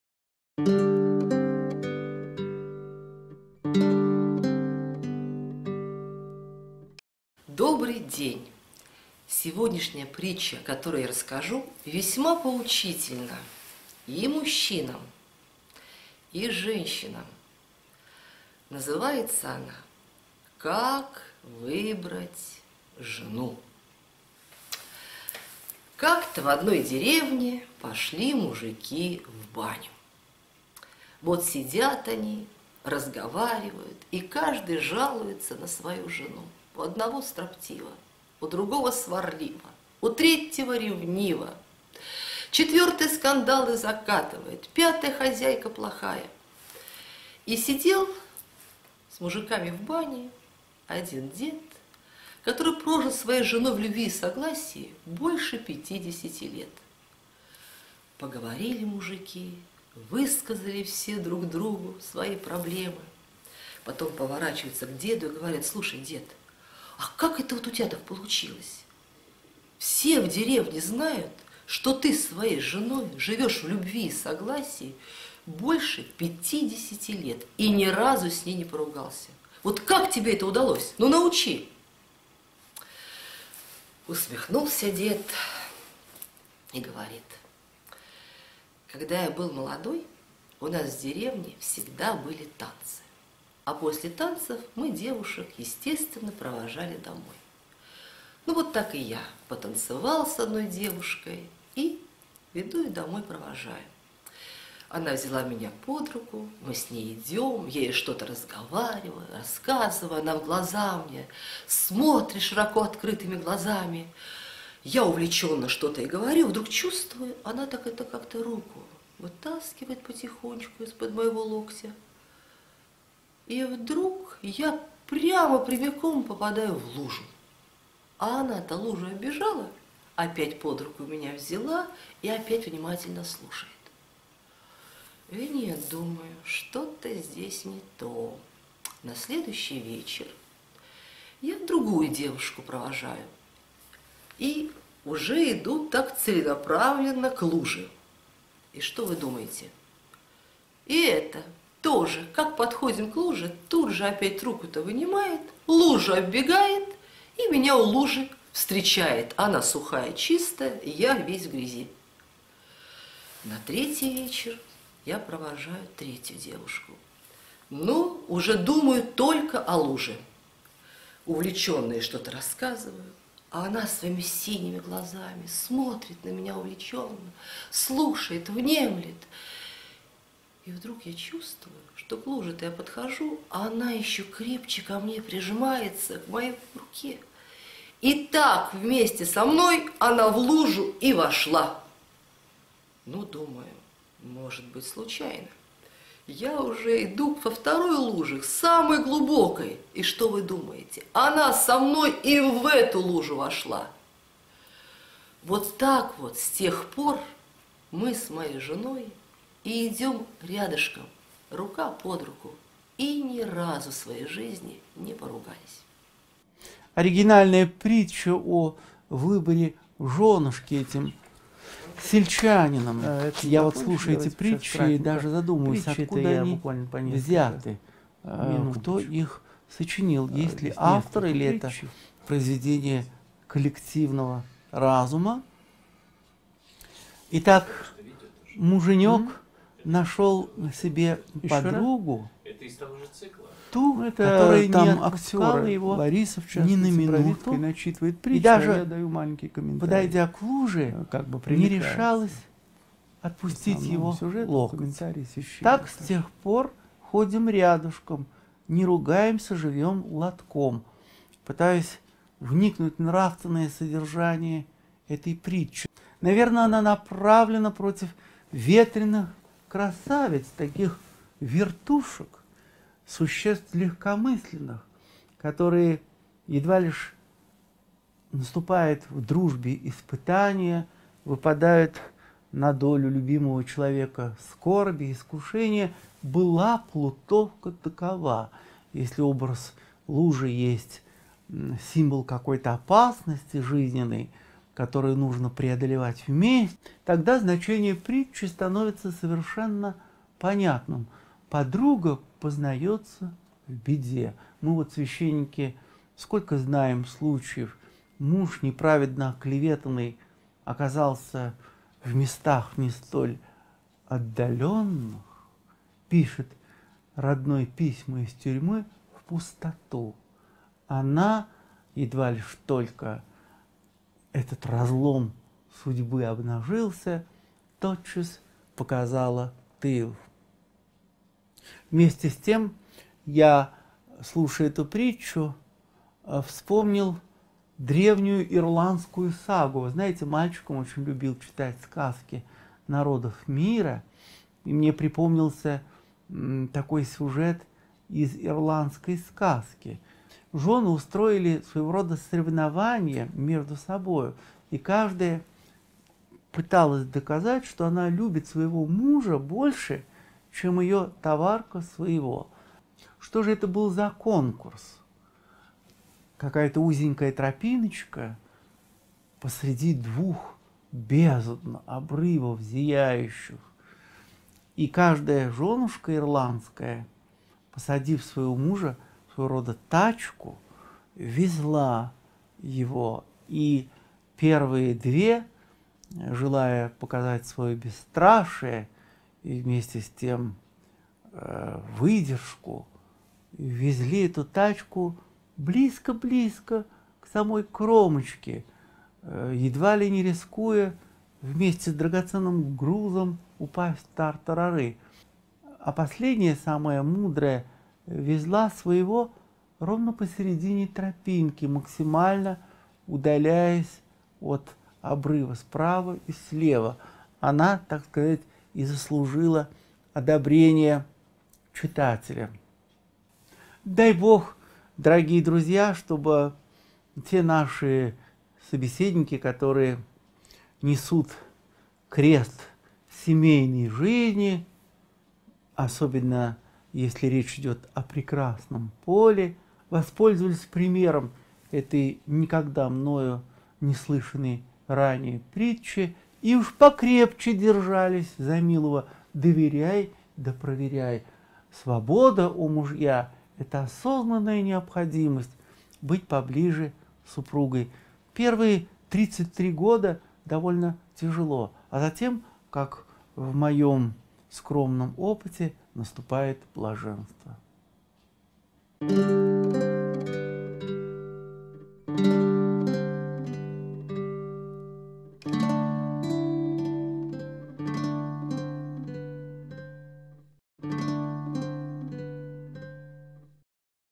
Как выбрать жену - Крылатые Притчи - Лариса Бравицкая & о. Артемий Владимиров
Однажды мужики спросили деда, скажи вот ты с женой живешь полста лет и не ругаетесь (в деревне все про всех знают). Как это?

- Вы знаете, что молодые вечерами ходят на посиделки, а потом провожают парни под ручку девок, по центральной улице гуляют. Один вечер проводил - ничего, два - повод... а три считай жених. Вот и я пошел провожать одну, иду что-то говорю, а она вдруг стала вытаскивать потихоньку свою руку из под моей. Я не понял, оказывается я шел прямо в лужу на дороге, сворачивать не стал. Она лужу обежала и опять меня под руку. К следующей луже я шел целенаправленно. Она также убирала руку.

На следующий вечер с другой девушкой я по прежнему маршруту. Та же картина, обегала лужи. На следующий вечер пошел с третьей. И опять посередине дороги по лужам. Подхожу - она за меня крепко держится, слушает меня и... пошла по луже со мной. Ага - просто не увидела лужу. Тогда я к следующей - поглубже. Подруга ноль внимания на лужу. Вот с тех пор и ходим рядышком и не ругаемся, живем ладком.

Лариса Бравицкая & о. Артемий Владимиров